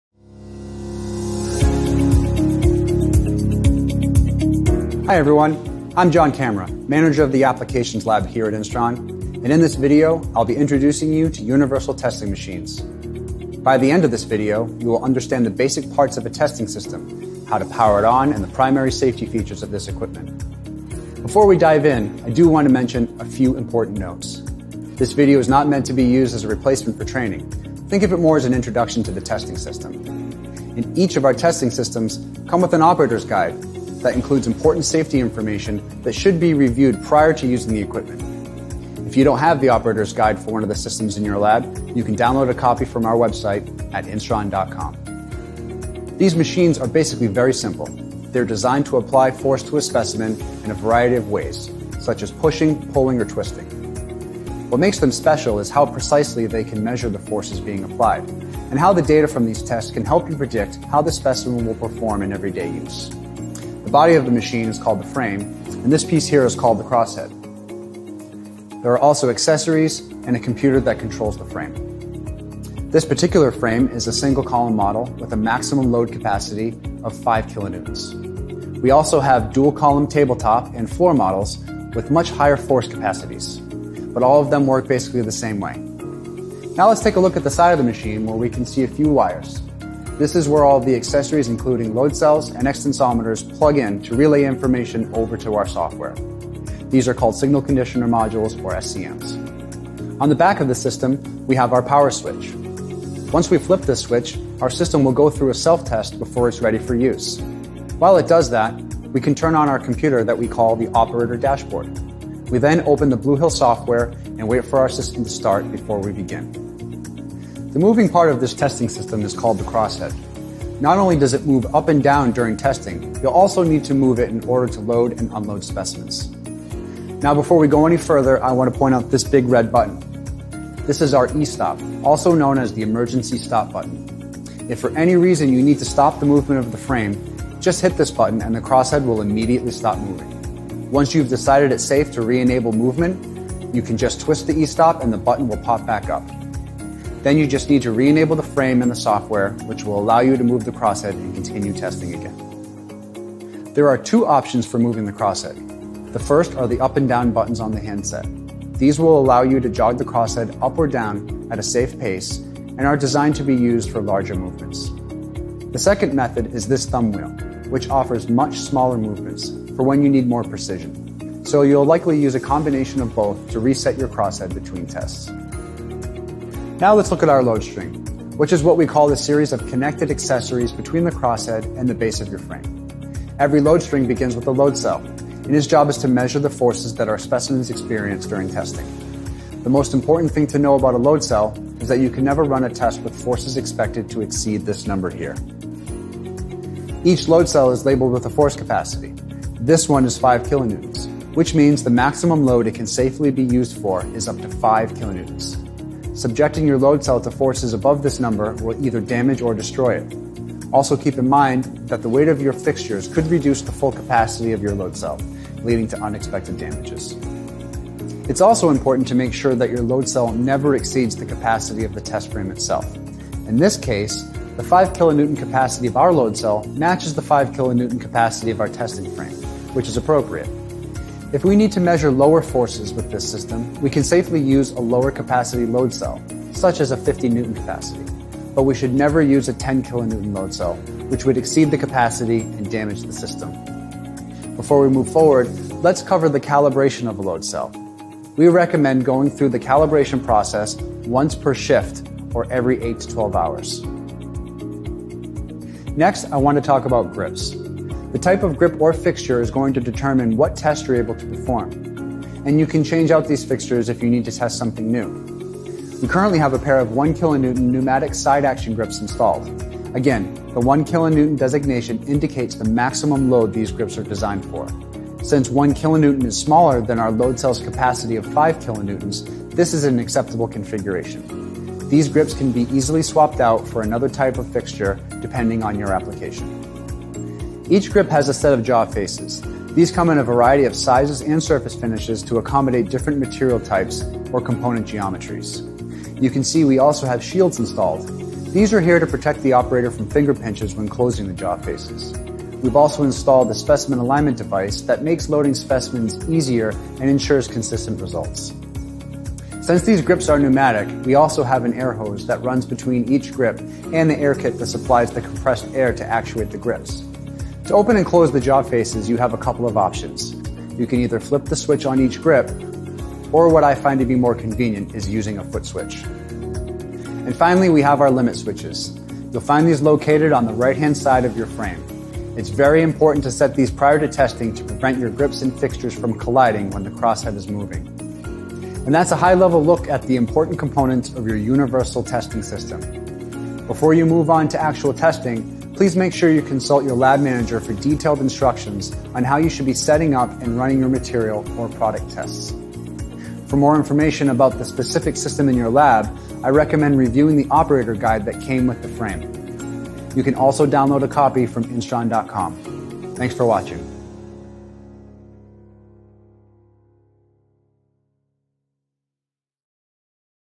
Introduction to Instron® Universal Testing Machines
Instron universal testing machines are designed to perform tensile, compression, flex, peel, puncture, friction, shear tests, and more.

This video provides a basic introduction to using an Instron universal testing machine. You will learn the basic parts of the testing system, how to power on the machine, and the primary safety features of the equipment.